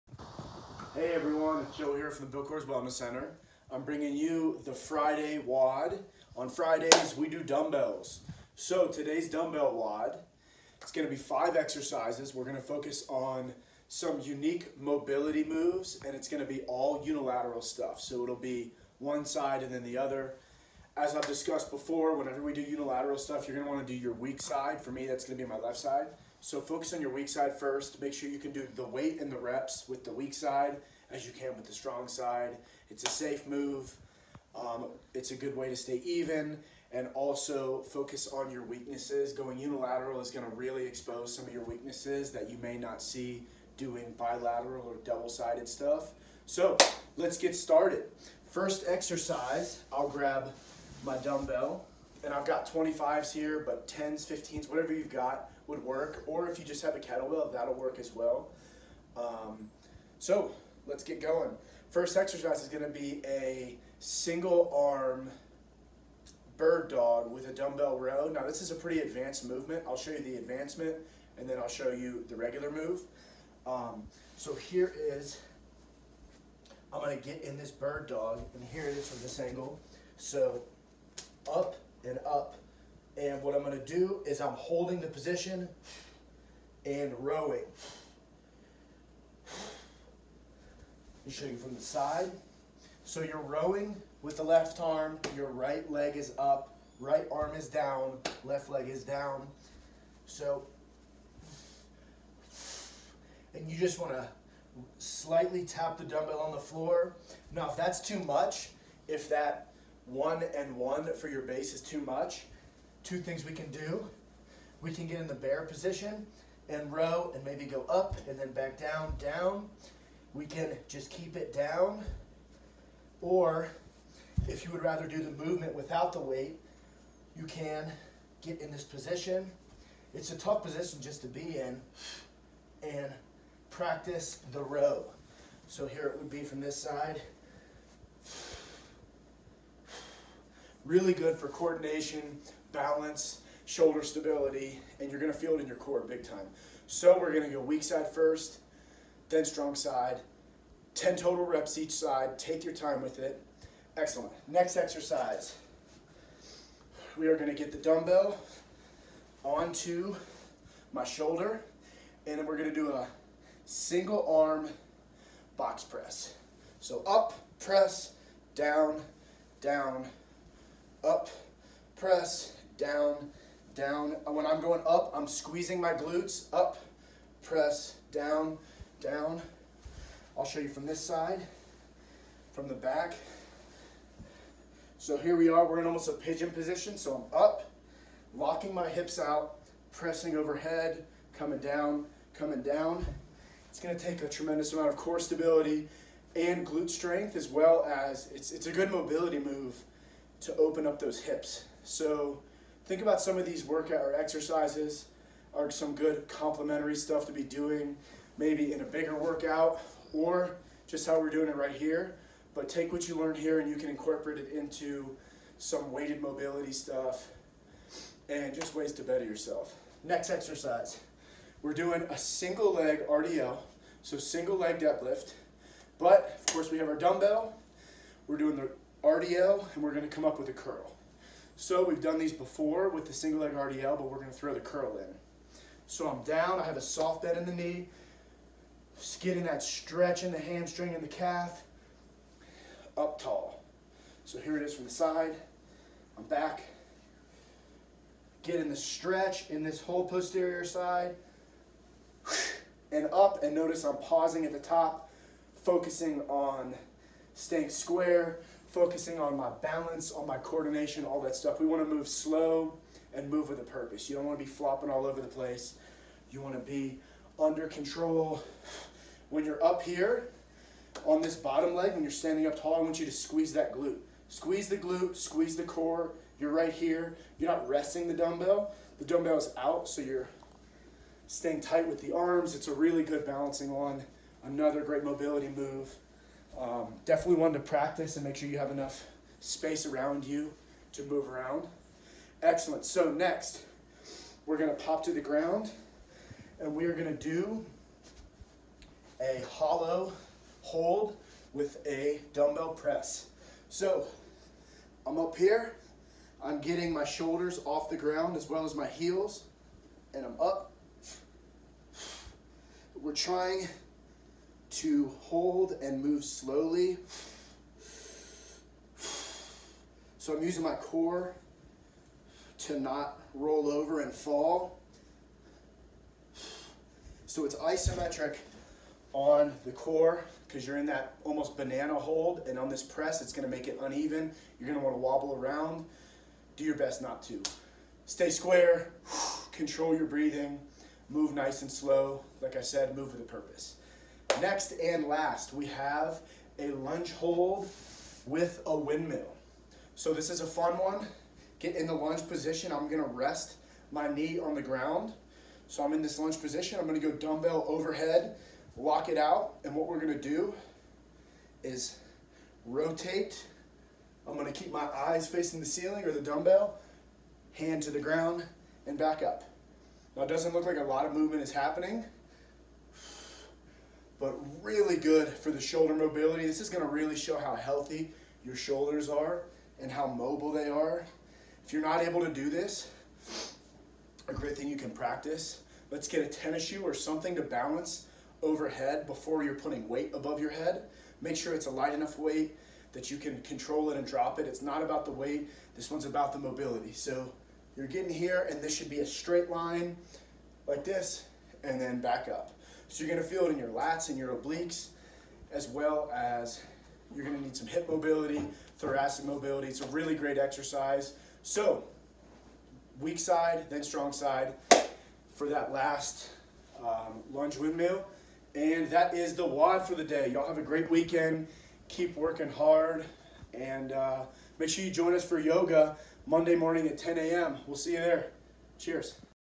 Dumbbell Mobility Unilateral WOD
What a great way to start off the weekend! With some fun mobility moves that will not only challenge your strength but your core & balance as well!

This would be a GREAT workout to knockout outside in the grass in your bare feet!

1. Single arm bird dog DB row x10
2. Single arm box press x10
3. SLRDL + DB curl x10
4. Hallow hold DB press x10
5. Lunge hold DB windmill x10

Be safe with these moves and have a GREAT weekend! Cheers to your health.